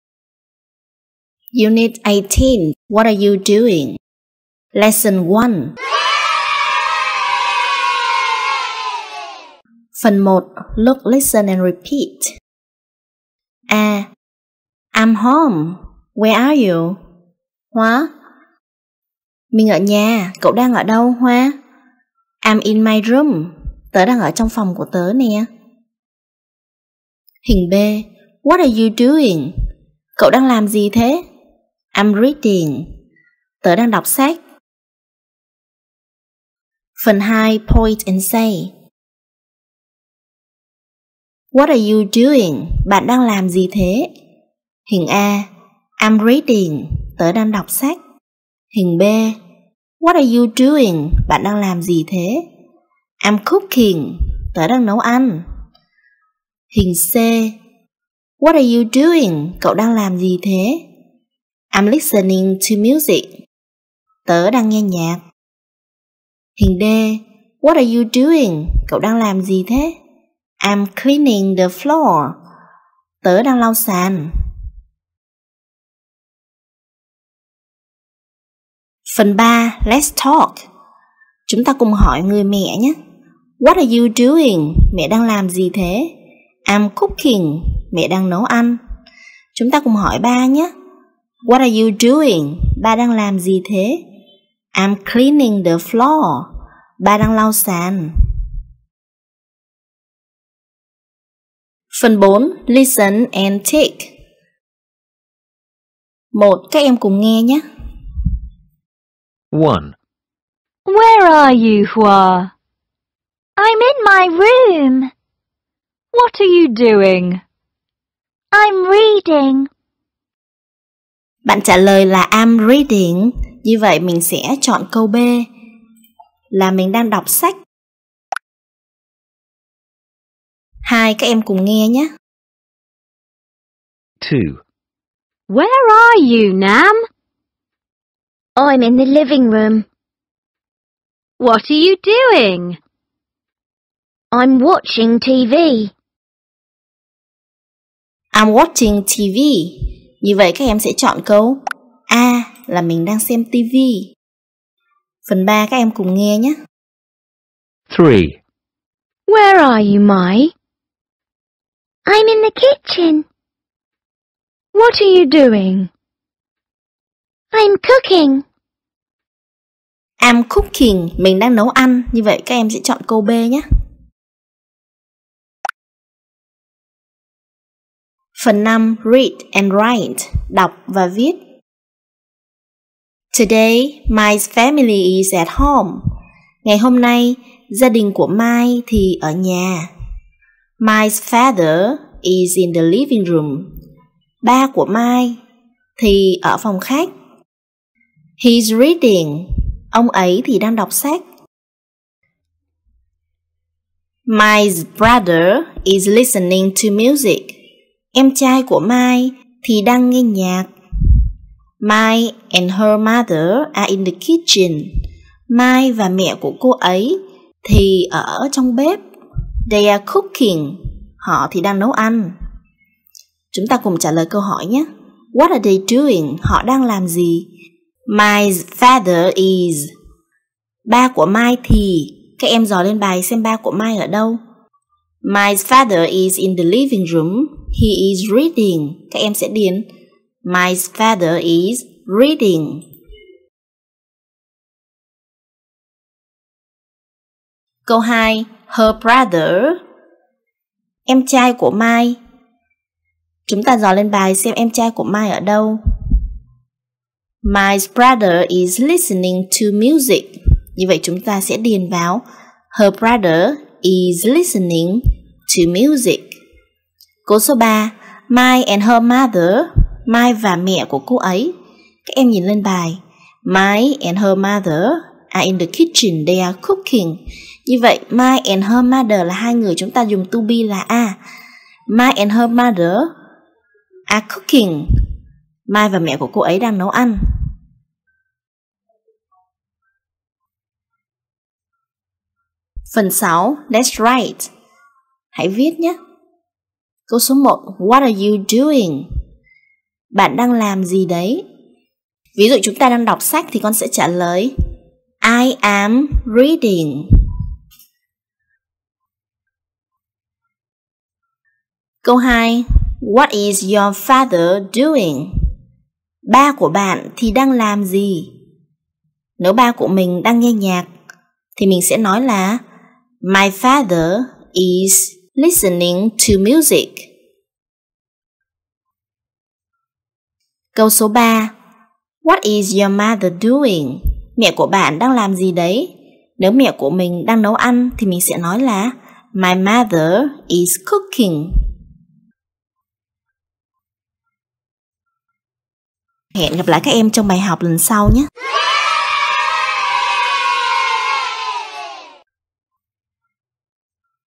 Tiếng Anh lớp 3 - Unit 18 Lesson 1: What are you doing?
Xin chào các em!
Các em cùng học từ vựng và các cấu trúc trong chương trình Tiếng Anh lớp 3 với cô nhé.

Hôm nay chúng ta cùng học Unit 18 lesson 1: What are you doing? nhé các em.

Trong bài, về TỪ VỰNG các em sẽ được học thêm các từ mới về các hoạt động như là: cook, listen, skate, sing, dance, play, watch,.

Về NGỮ PHÁP các em học thì hiện tại tiếp diễn : Tobe + V_ing để diễn tả một người đang làm việc gì:
I'm cooking.
She is watching TV.
Phong and Hoa are skating.

Và khi muốn hỏi bạn đang làm gì mình dùng câu:
Anh ấy/ cô ấy đang làm gì mình dùng câu:
What is he/she doing?

Các em nhớ học từ vựng để mình tích luỹ từ và sử dụng cho các bài học sau và để sau mình học các chương trình Tiếng Anh chuyên sâu hơn nhé.

Chúc các em học tốt.